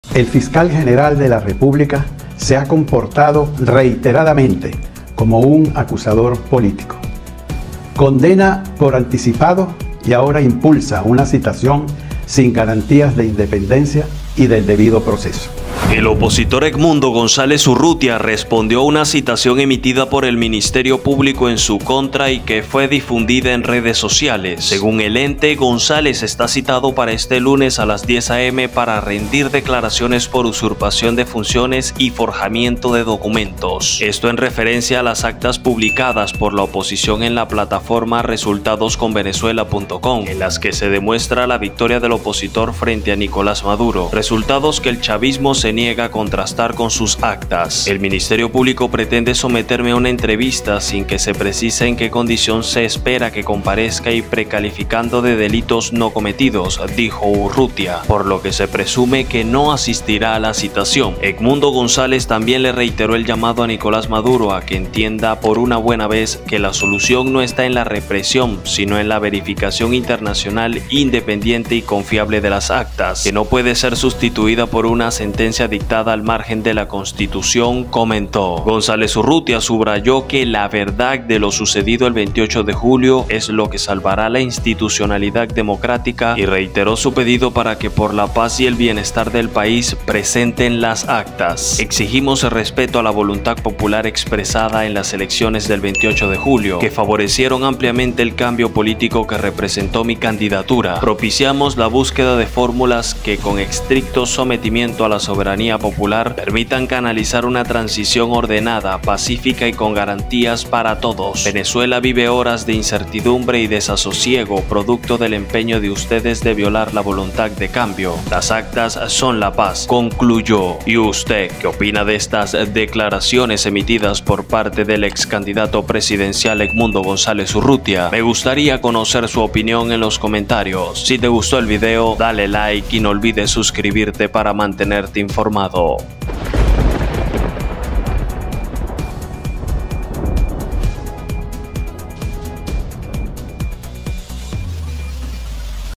EDMUNDO GONZÁLEZ Responde a CITACIÓN de la FISCALÍA
Edmundo González Urrutia cuestionó la citación de la Fiscalía Venezolana en su contra por falta de garantías de independencia

El abanderado de la oposición mayoritaria de Venezuela, Edmundo González Urrutia, dijo este domingo que la citación del Ministerio Público (MP, Fiscalía) afecto al chavismo, para que comparezca este lunes, como parte de una investigación por supuesta “conspiración” y otros delitos, carece de “garantías de independencia y del debido proceso”.